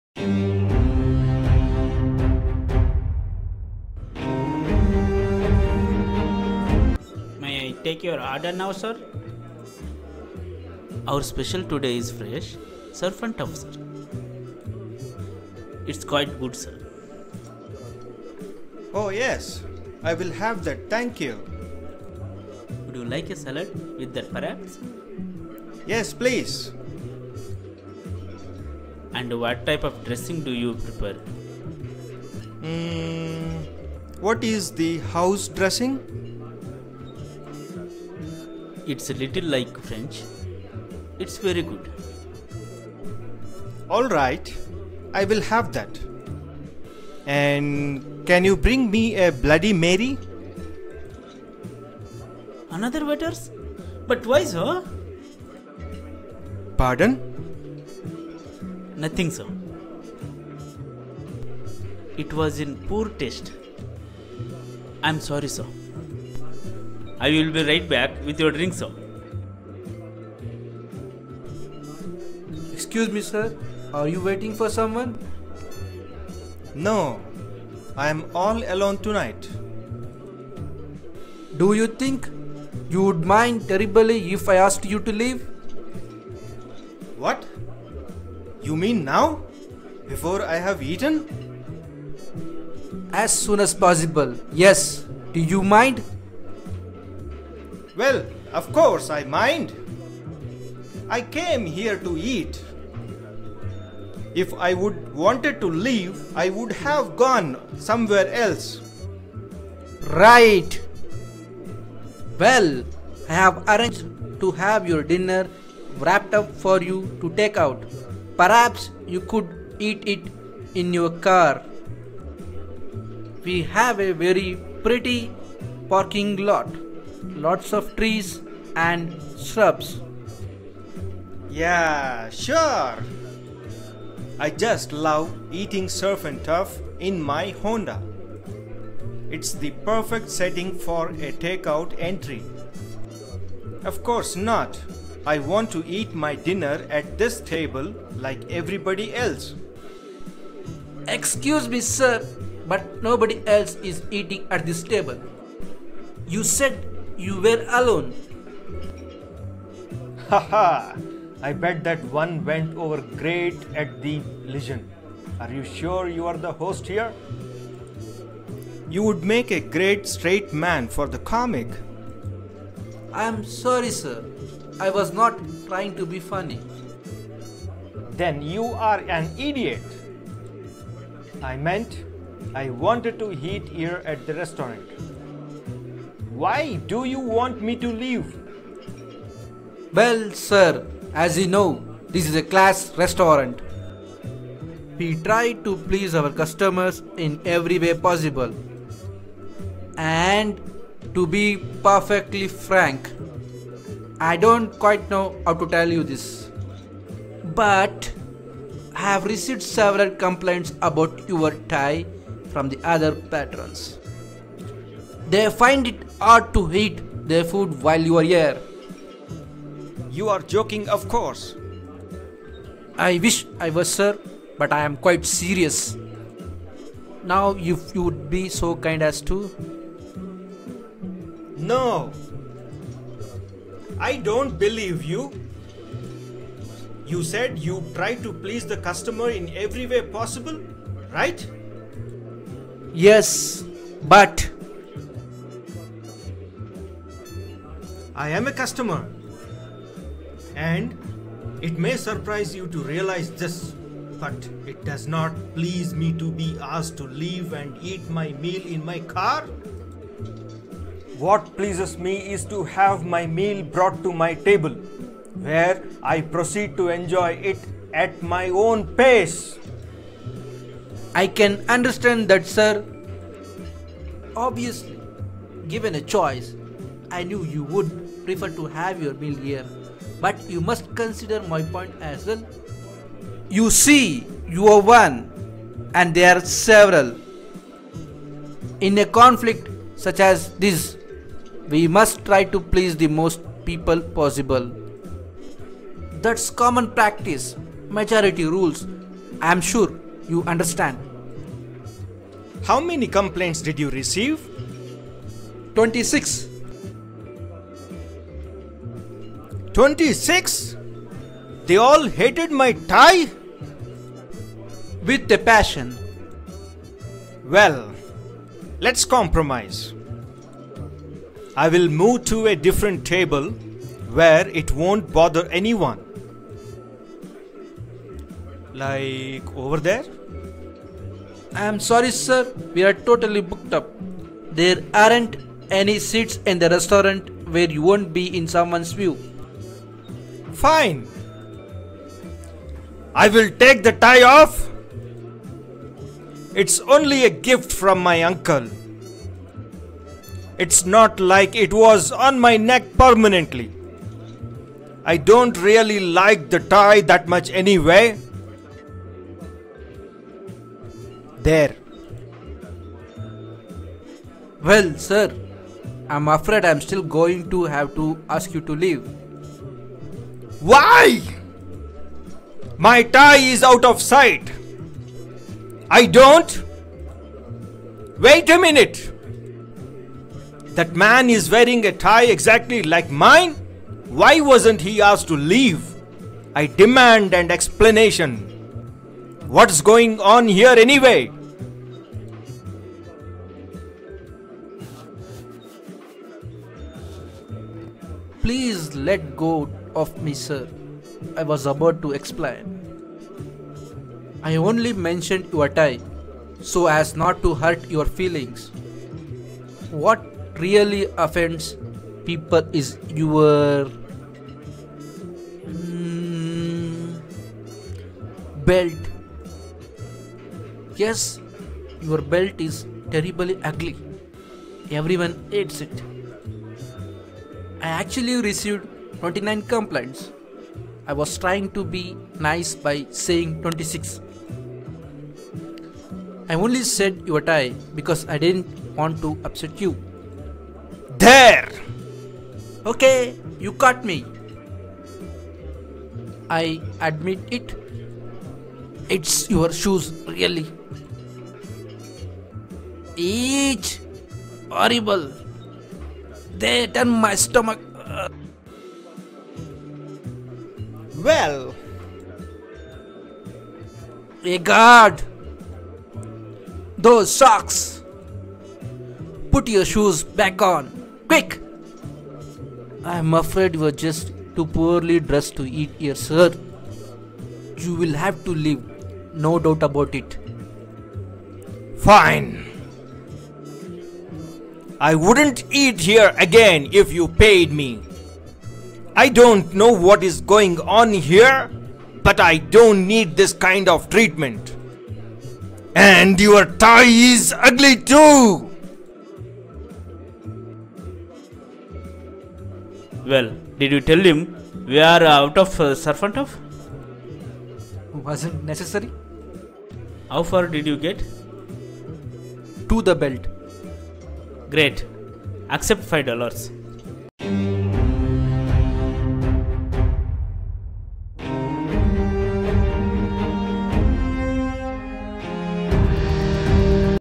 The Tie | cartoon | vishwa sree yadla
The Tie cartoon movie is depicting the tie-wearing the customer